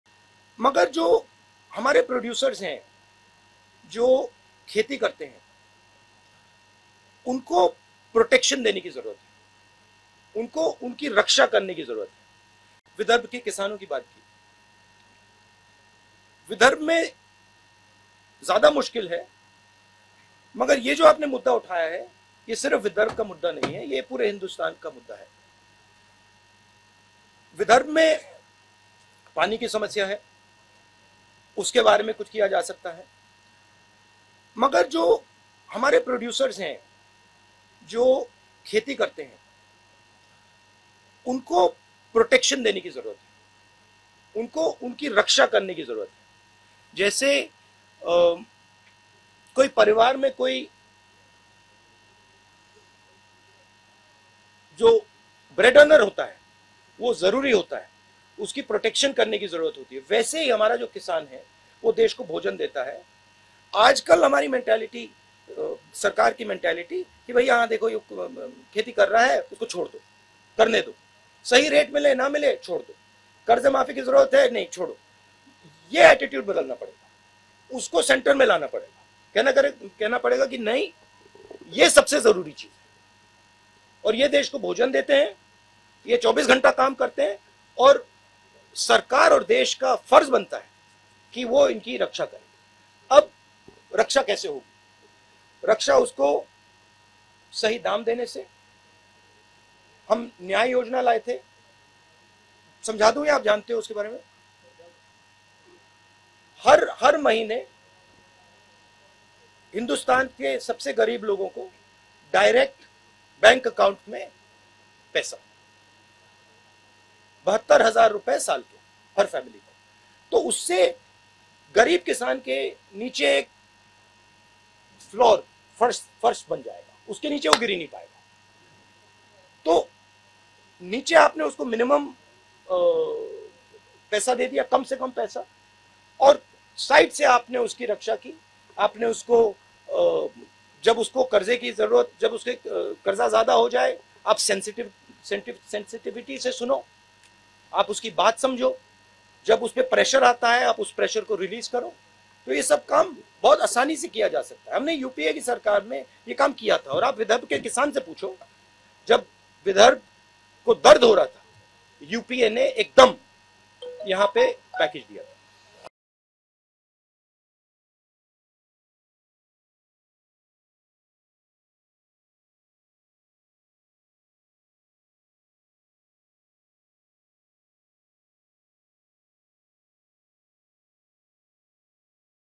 Rahul Gandhi : शेतकऱ्यांच्या प्रश्नावर राहुल गांधी काय म्हणाले... | LetsUpp Marathi
राहुल गांधी यांच्या नेतृत्वाखाली निघालेली भारत-जोडो यात्रा विदर्भात आहे. पत्रकार परिषदेत राहुल गांधी यांनी शेतकरी आत्महत्येच्या प्रश्नावर उत्तर देताना त्यांचा शेतकऱ्यांसाठीचा प्लॅन सांगितला. गरीब शेतकऱ्यांना कशी मदत केली पाहिजे हे गांधी यांनी मांडले आहे.